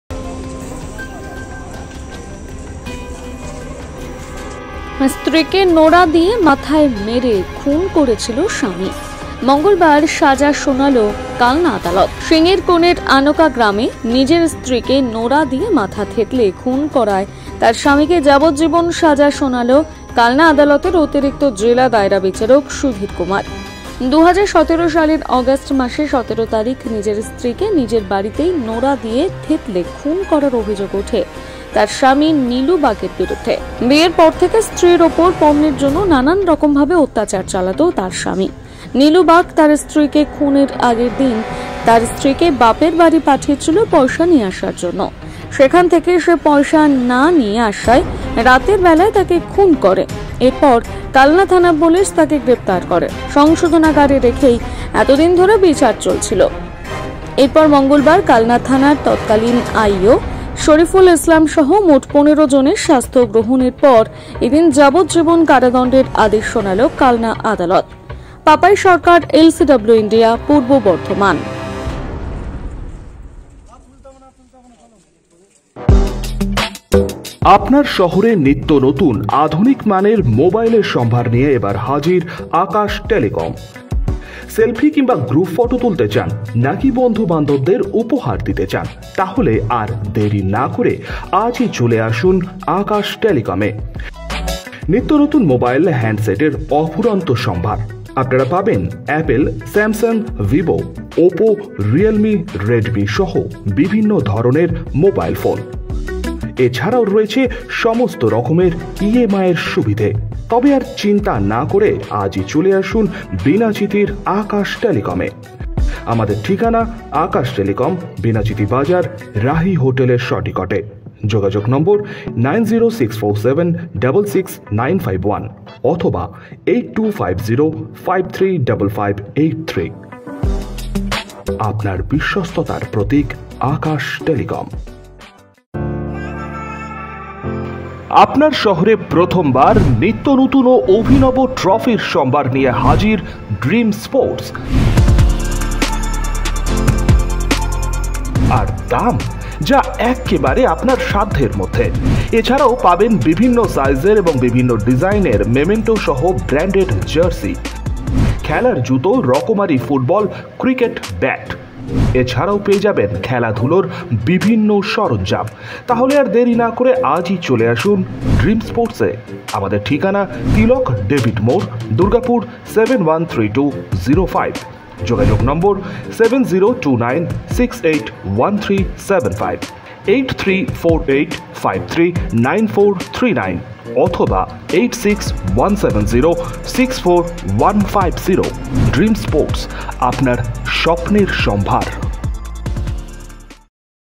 স্ত্রীকে নোড়া দিয়ে খুনের অভিযোগ, যাবজ্জীবন কারাদণ্ডাদেশ আদালতের
স্ত্রীকে নোড়া দিয়ে খুনের অভিযোগ,
যাবজ্জীবন কারাদণ্ডাদেশ আদালতের